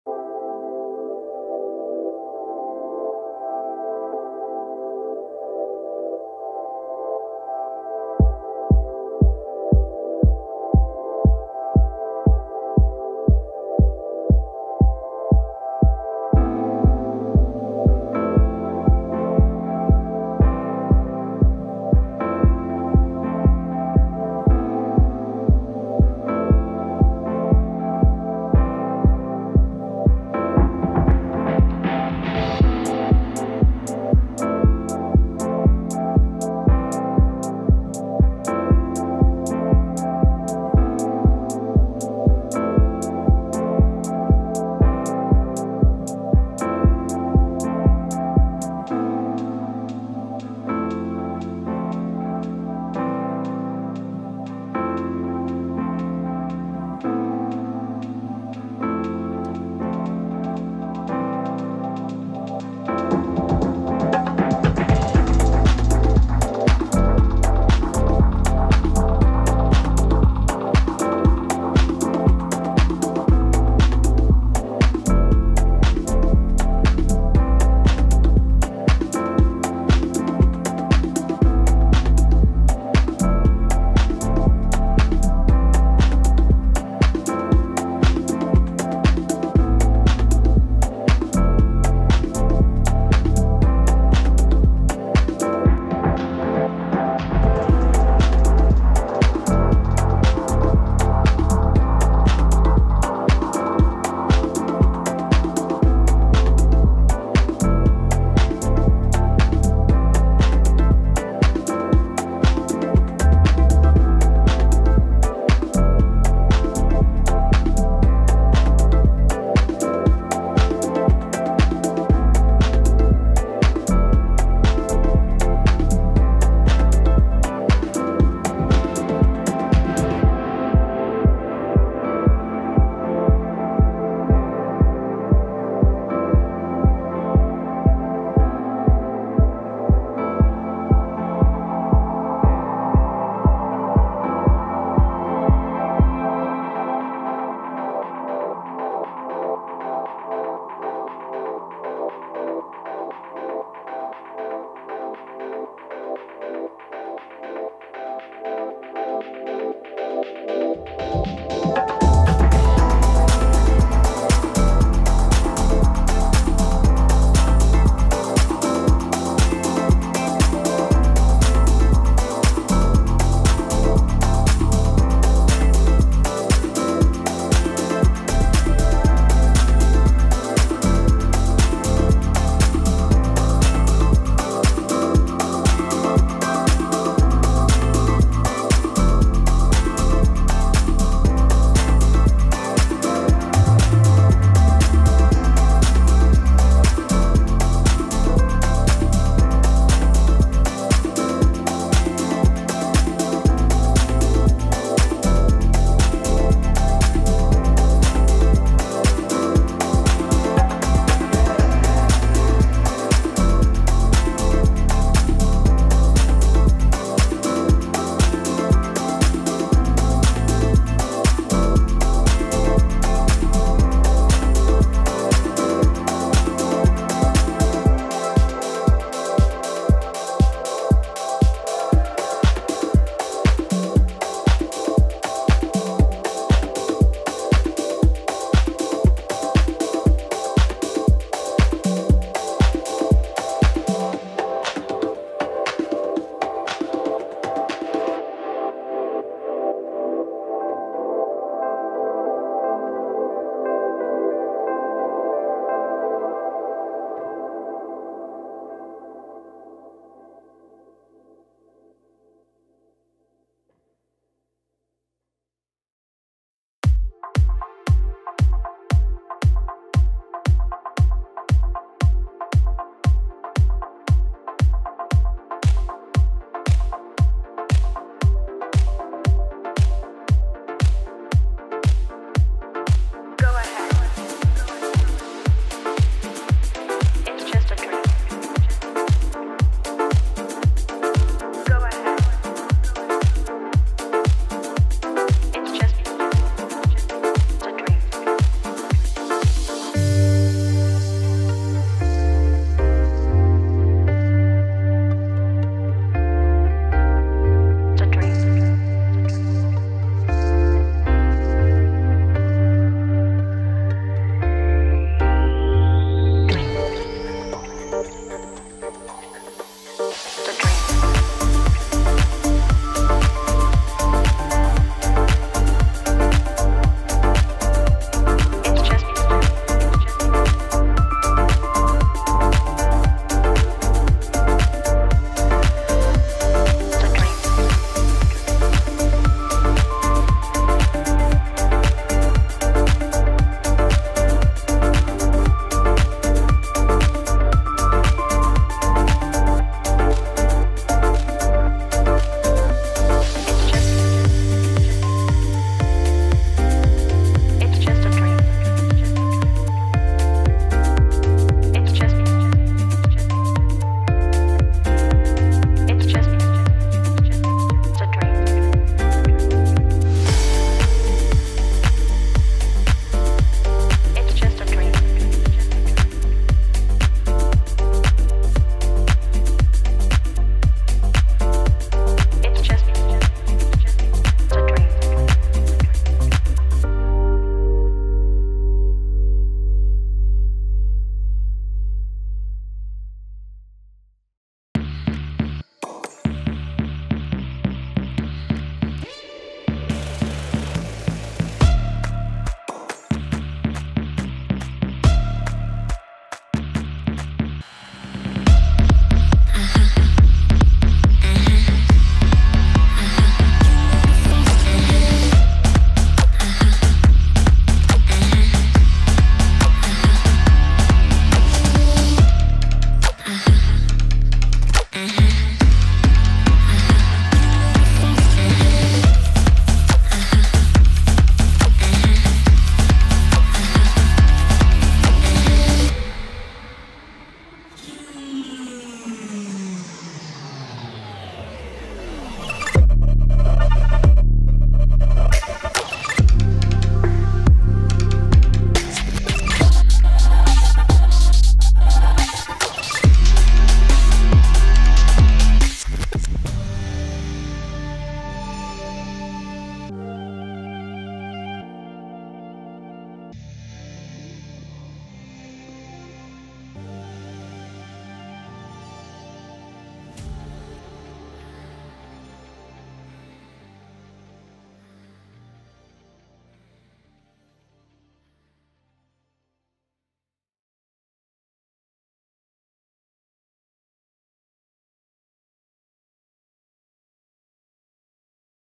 This guy FPV Dji Avata - Cinematic Fpv
here at waterloo park doing some slow flying mixed with cinematic stuff enjoy